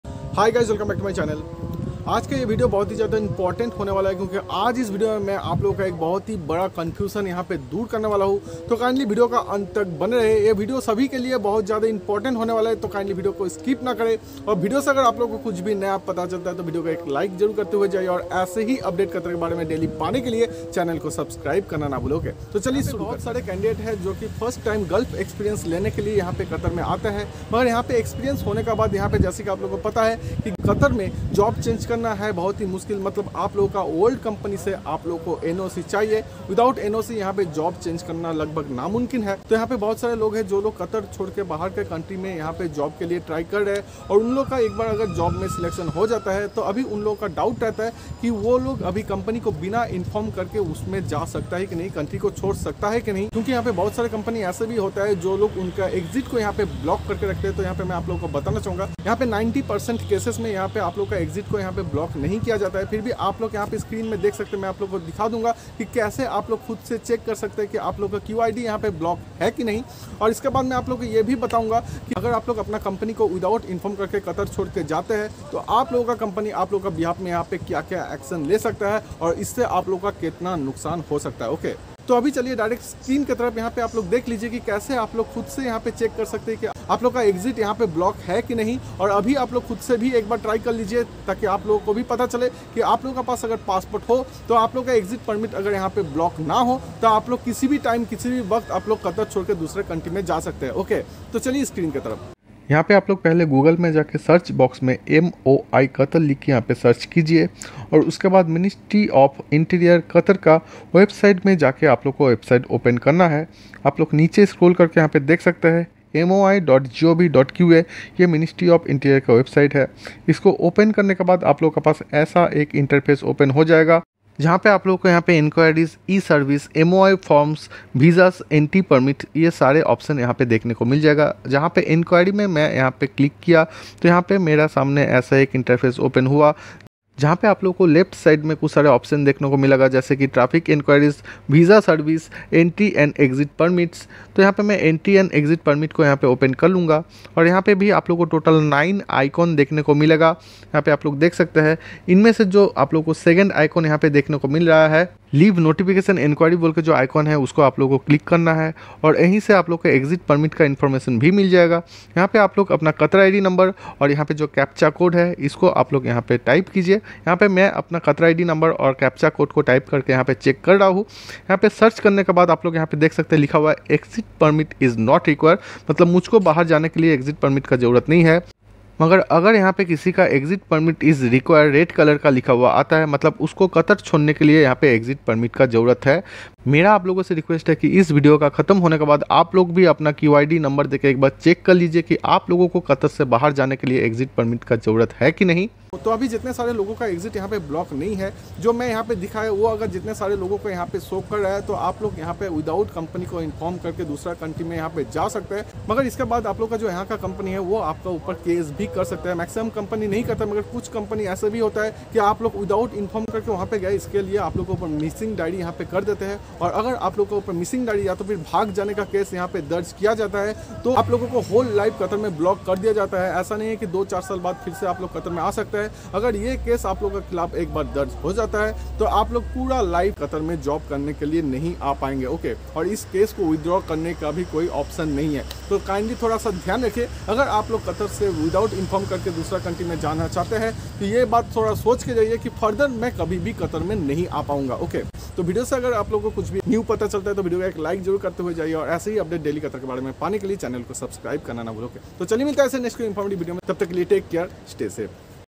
💥Qatar Exit Permit Update| Company को बिना बताये घर जा सकते है| How To Check
In this video I am sharing qatar exit permit related important information.